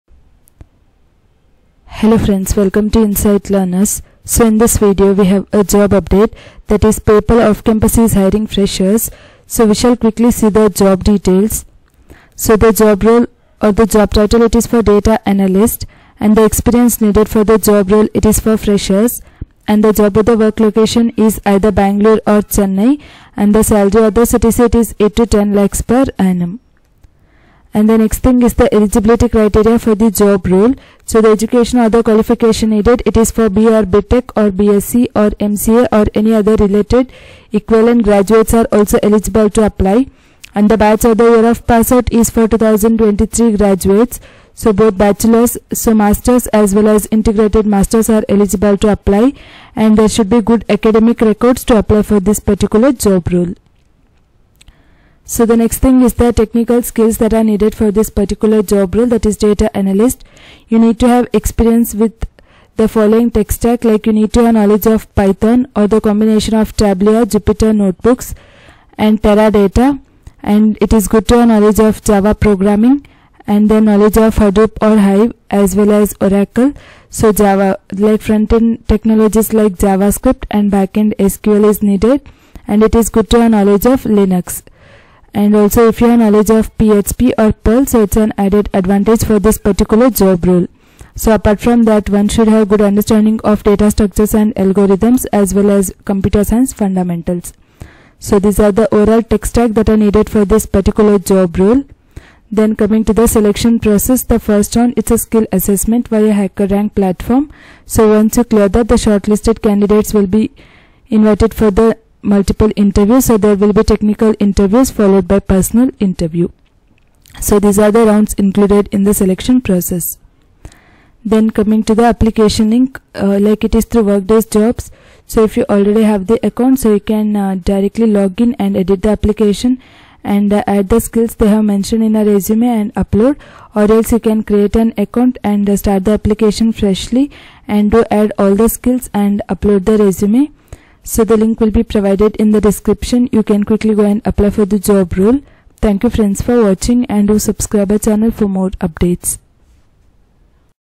PayPal Off Campus Hiring Graduates for the Role of Data Analyst | Software | Freshers
Job Role: Data Analyst
Experience: Freshers

About the Company:
# PayPal Holdings, Inc. is an American multinational financial technology company operating an online payments system in the majority of countries that support online money transfers and serves as an electronic alternative to traditional paper methods such as checks and money orders.
# The company operates as a payment processor for online vendors, auction sites and many other commercial users.
# PayPal's services allow people to make financial transactions online by granting the ability to transfer funds electronically between individuals and businesses.
# PayPal partially opened its platform, allowing other services to get access to more APIs and to use its infrastructure in order to enable peer-to-peer online transactions.